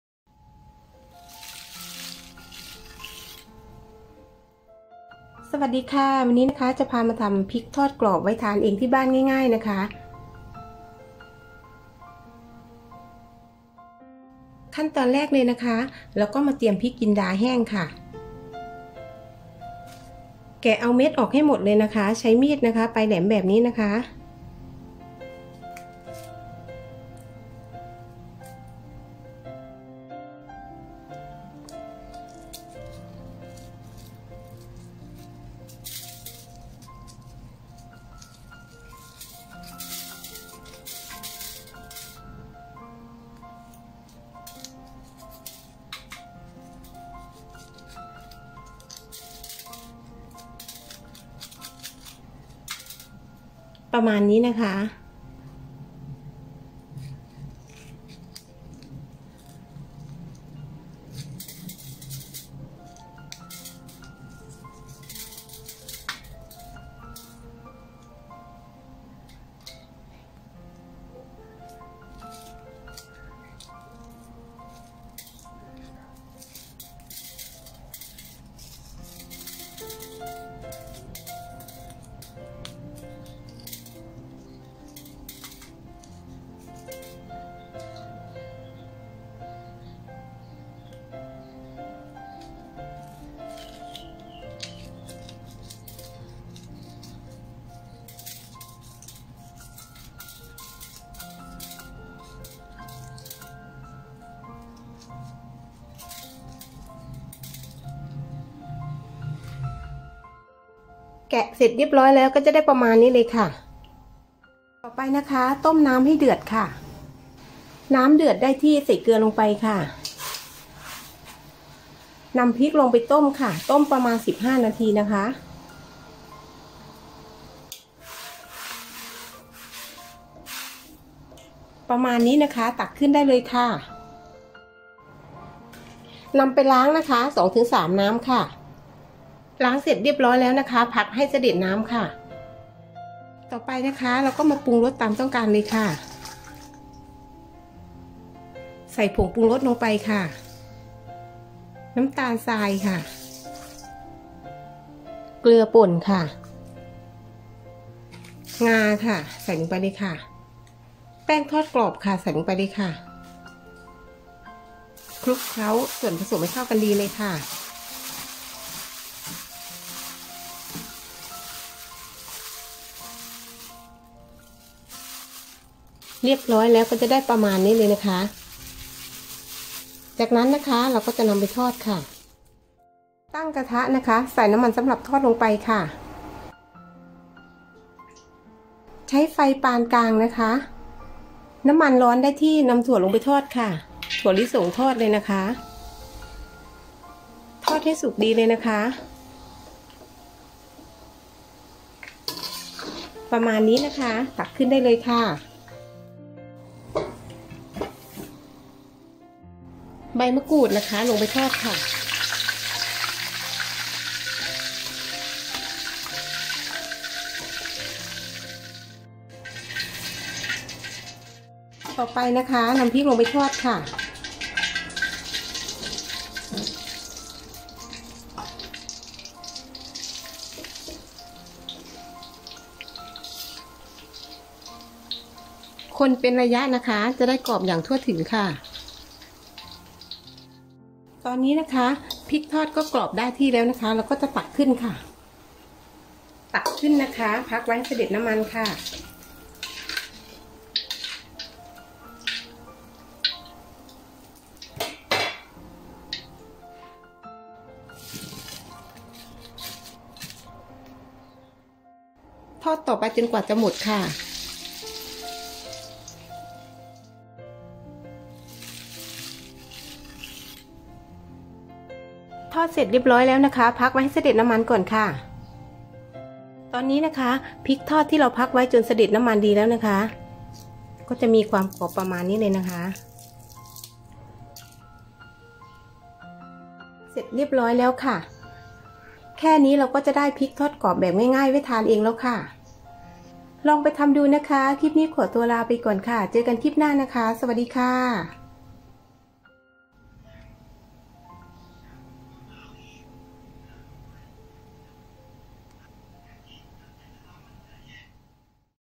พริกทอด lkate karakate
ส่วนผสมพริกทอด
พริกจินดาแห้ง  100 กรัม
แป้งทอดกรอบ 1/4 ถต
ผงปรุงรส 1/2 ชช
น้ำตาลทราย 1/2 ชช
เกลือป่น 1/4 ชช
งาขาว  2 ชต
ถั่วลิสง ตามชอบ
ใบมะกรูดตามชอบ